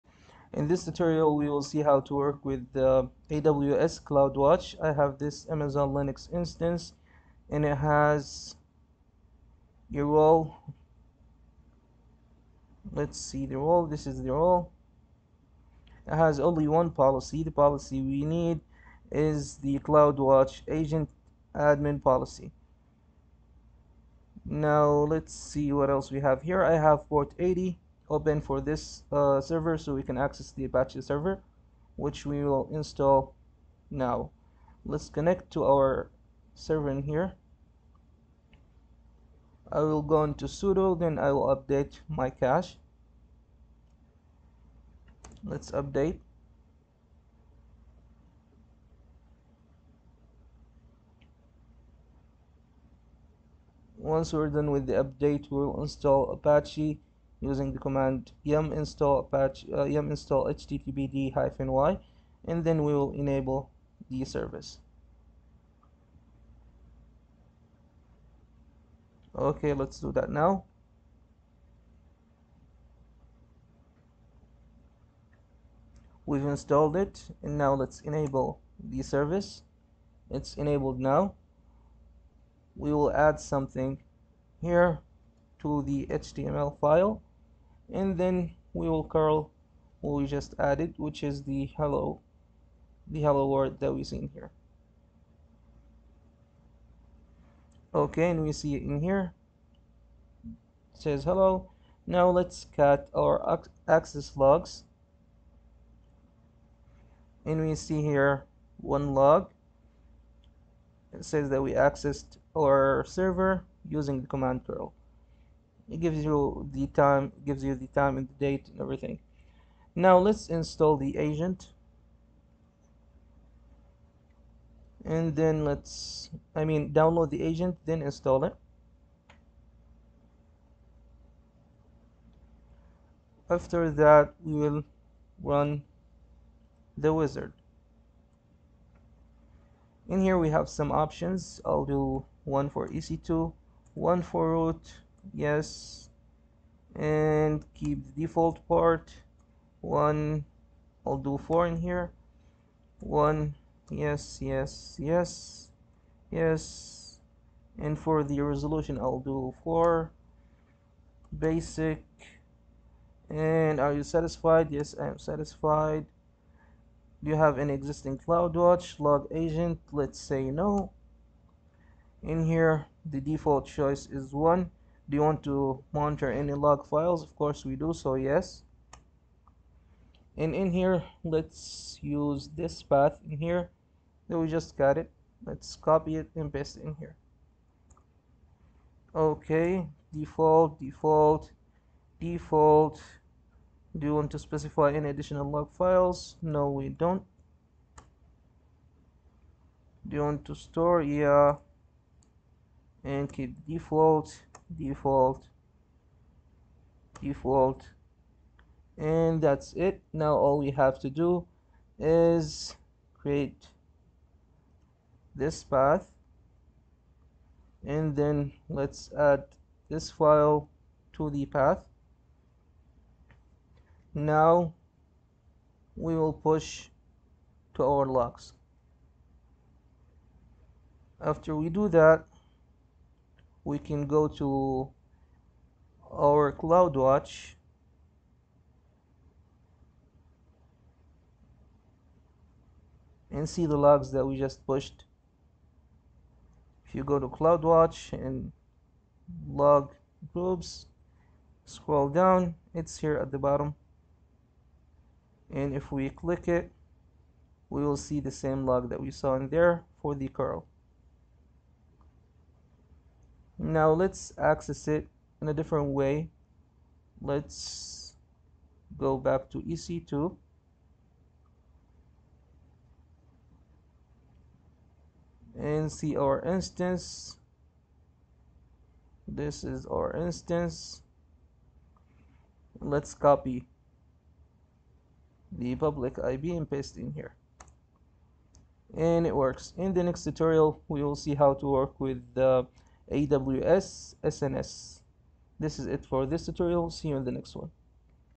How to Send Apache Access Log to AWS CloudWatch
Amazon CloudWatch is a monitoring and observability service built for DevOps engineers, developers, site reliability engineers (SREs), and IT managers. CloudWatch provides you with data and actionable insights to monitor your applications, respond to system-wide performance changes, optimize resource utilization, and get a unified view of operational health. CloudWatch collects monitoring and operational data in the form of logs, metrics, and events, providing you with a unified view of AWS resources, applications, and services that run on AWS and on-premises servers. You can use CloudWatch to detect anomalous behavior in your environments, set alarms, visualize logs and metrics side by side, take automated actions, troubleshoot issues, and discover insights to keep your applications
running smoothly.